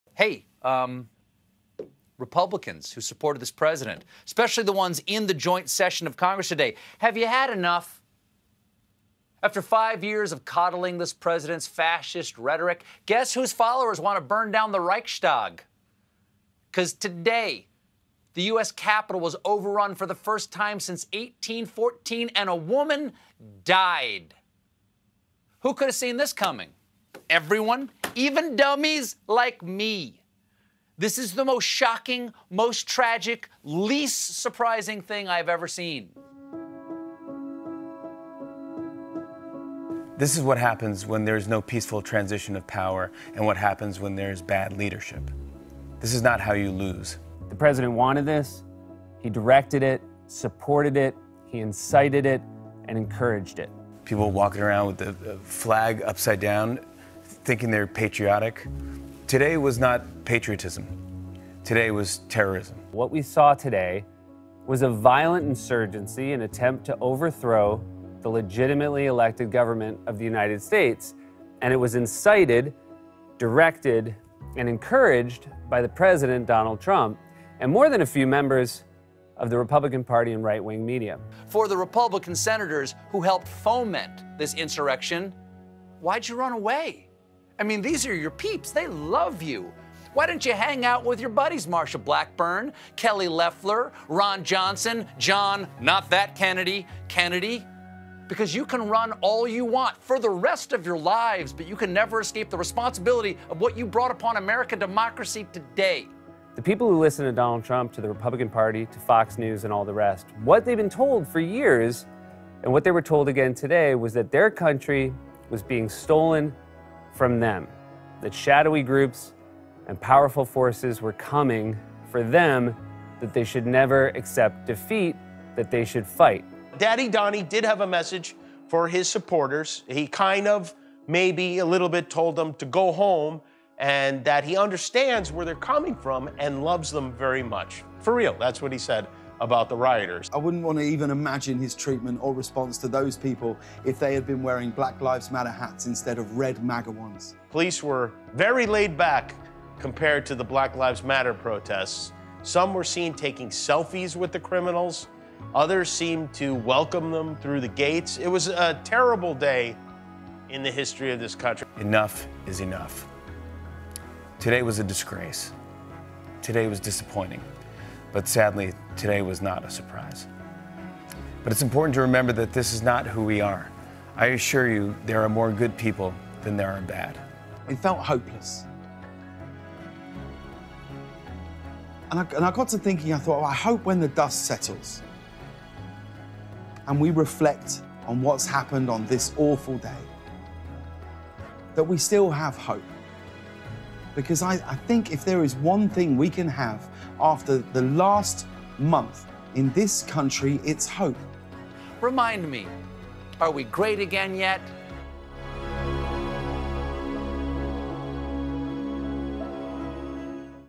Late Night TV Hosts React to Pro-Trump Capitol Riots | NowThis
Stephen Colbert, Jimmy Fallon, Seth Meyers, and other late night tv hosts are addressing and condemning the Pro-Trump Capitol riots of January 6.